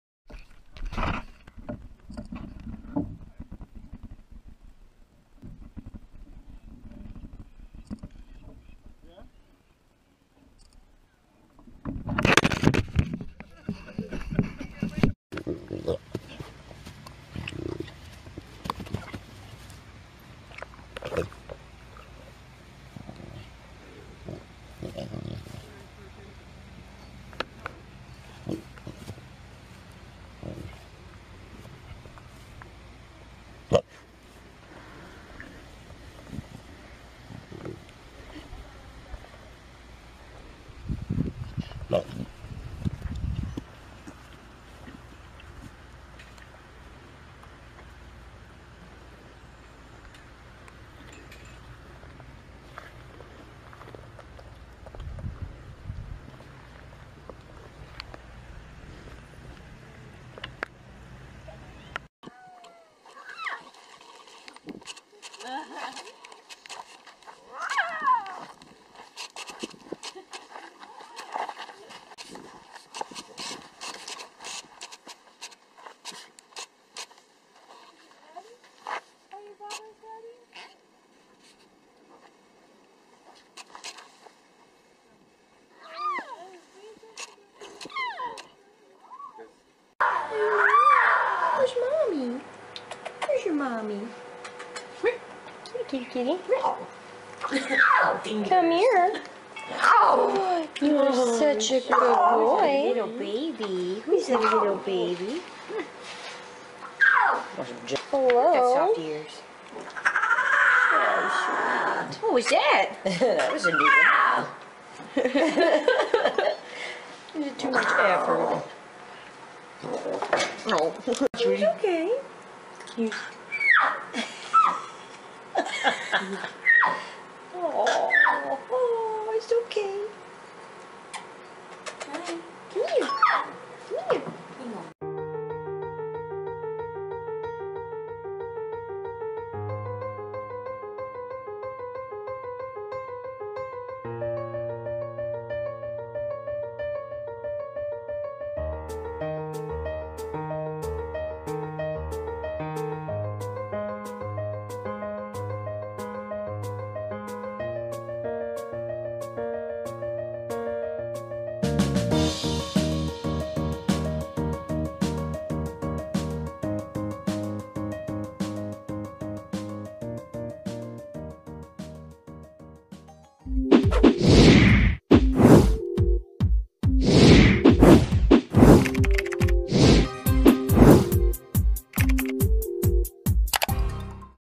Lion Cub Roar | Lion Cubs Roaring (Baby Edition) - Cutest Compilation (2021)
Enjoy this compilation of lion cubs roaring and squeaking. Lions are majestic creatures that roam Africa and they are the second-largest living cat species after the tiger.

Despite well-intentioned efforts, approximately 93% of wild lion populations have been killed or died in the last 50 years.